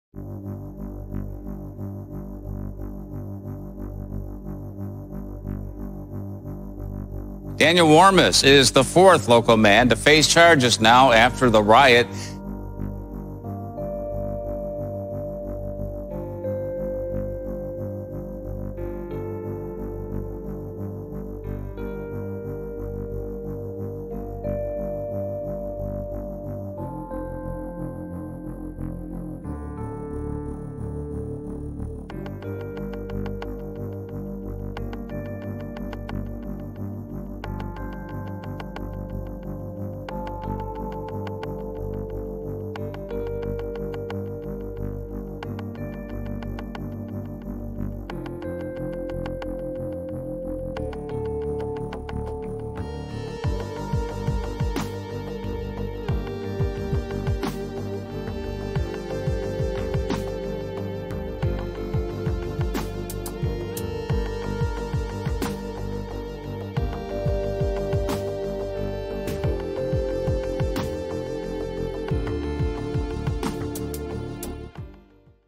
US Capitol Arrests: Daniel Warmus
Daniel Warmus was arrested for his participation in the January 6, 2021 attack and attempted insurrection at the United States Capitol.